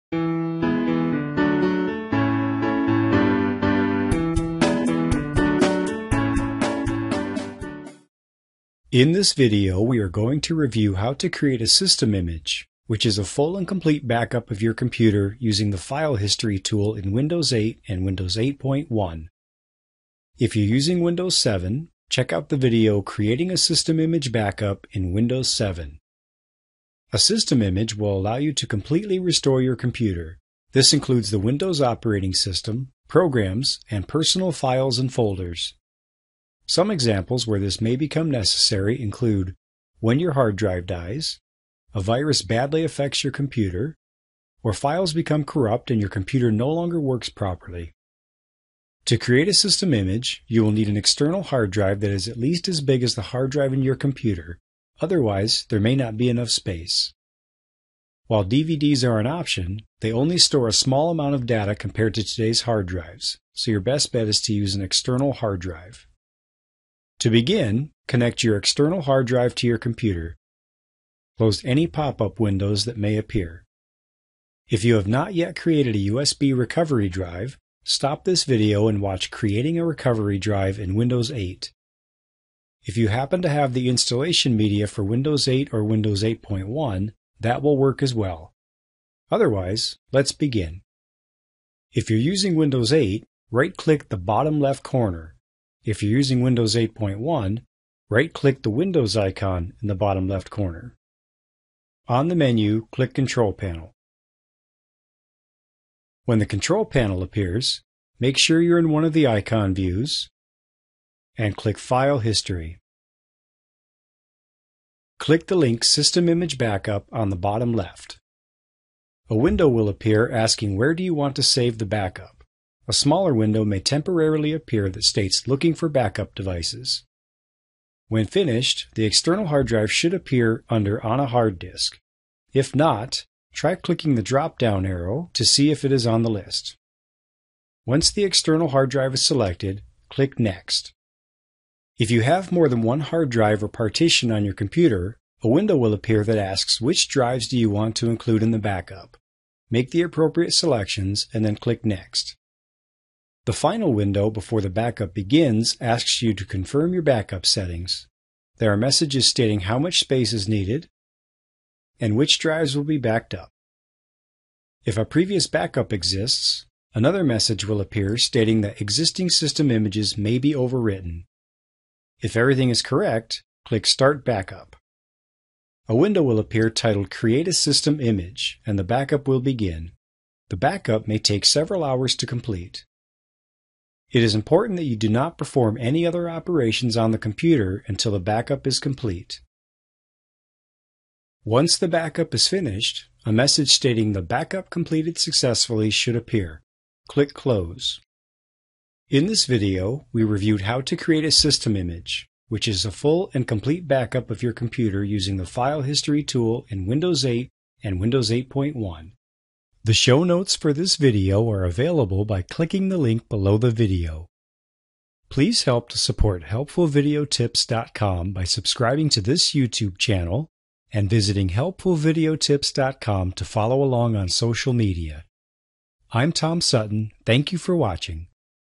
Creating a System Image Backup in Windows 8
Learn how to create a system image backup using the “File History” tool in Windows 8 and Windows 8.1.